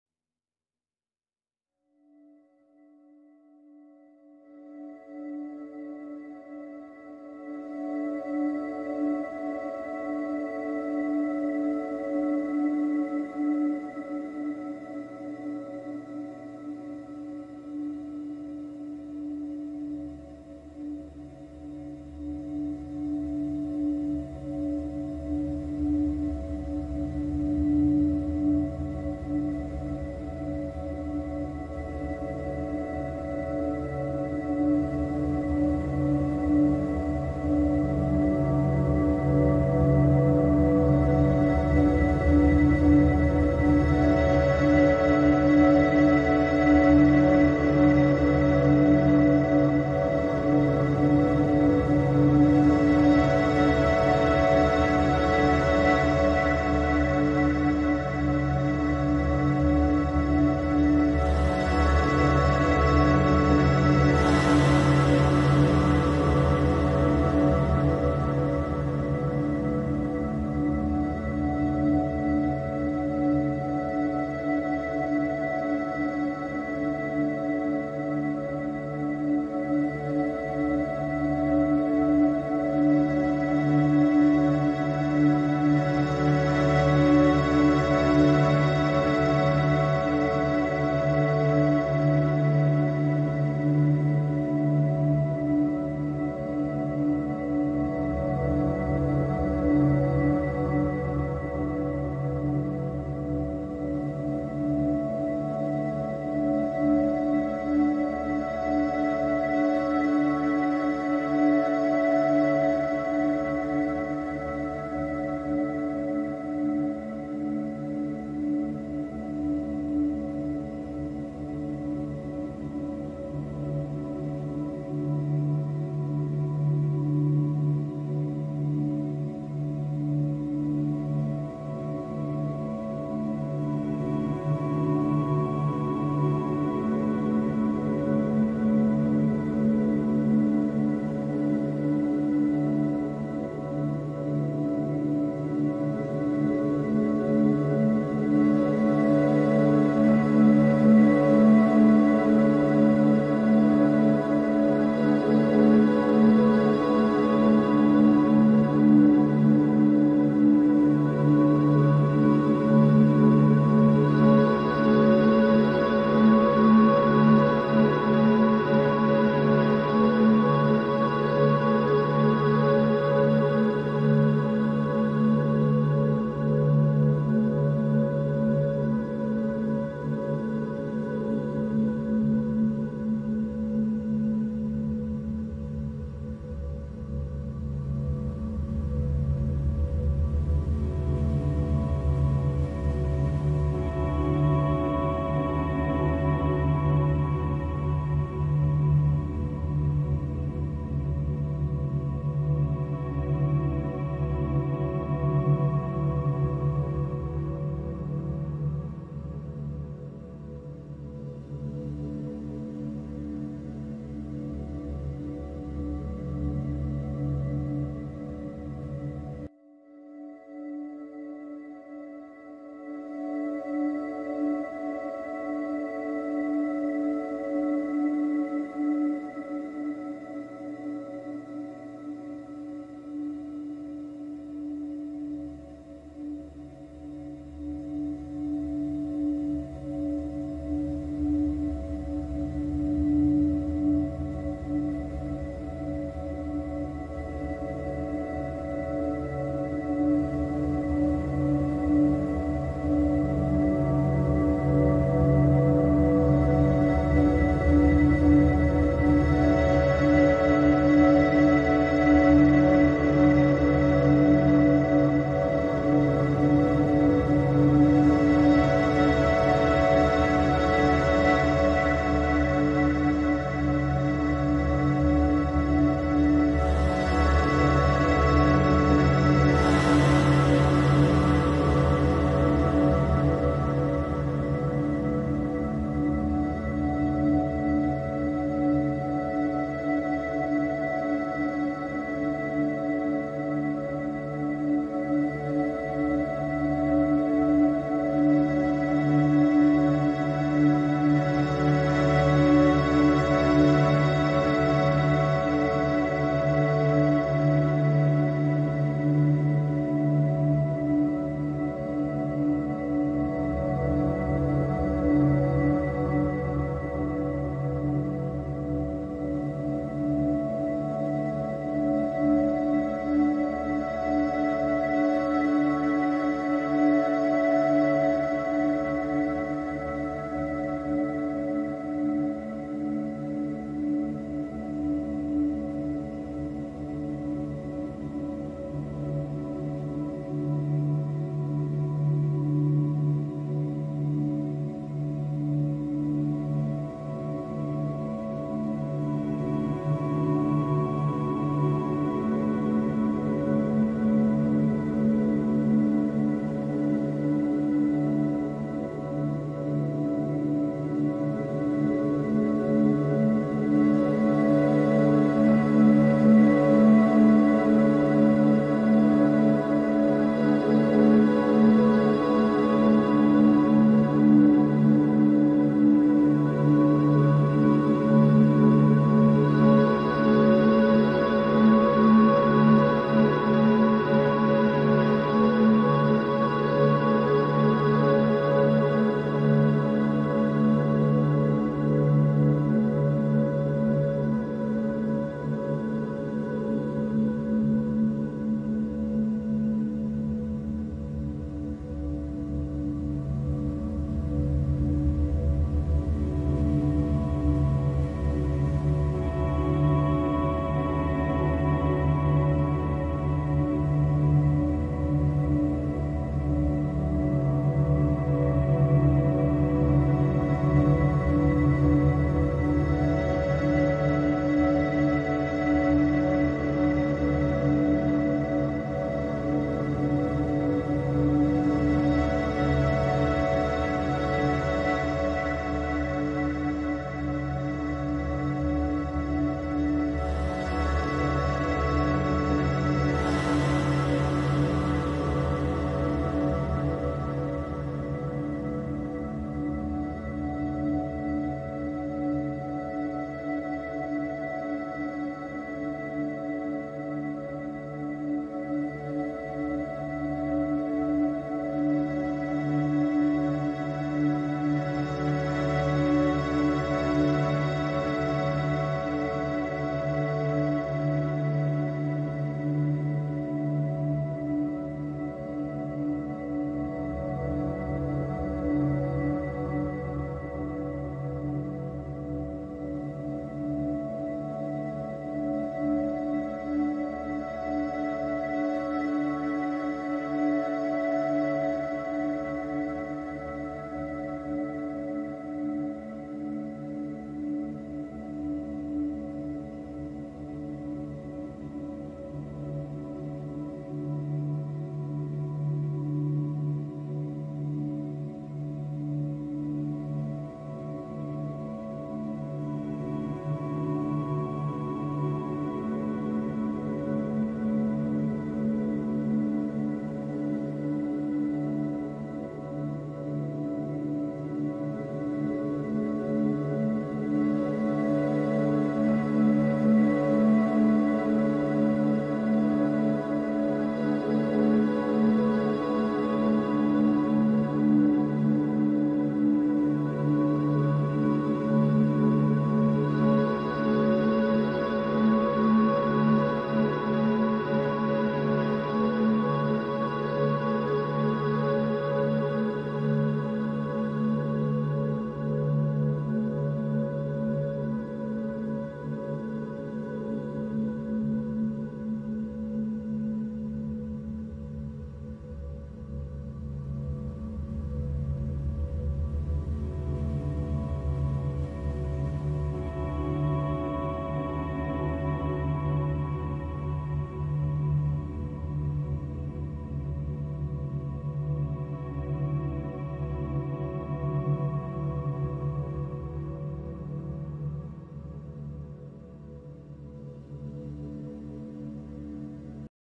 Meditating together | music for meditation
These videos and audio clips are for the consciousness of comfort, for meditation, yoga, calming, good and deep sleep, for relaxation after work. You can listen to the sounds of nature, the singing of birds, the sound of wind, the sound of rain, a waterfall, the rustle of leaves and so on